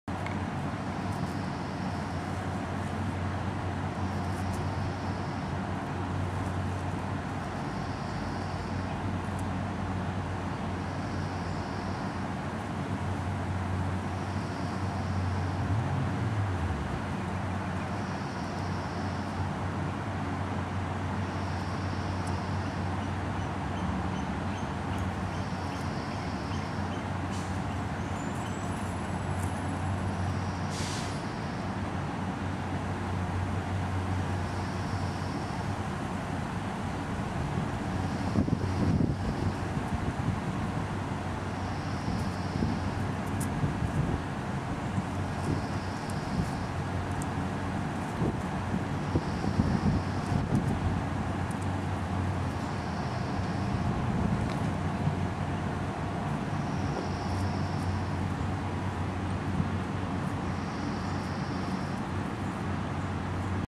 Chatting with NI
Unfortunately this video probably has to be viewed at full screen for you to actually see the squirrel. She is actually acting more timid than usual.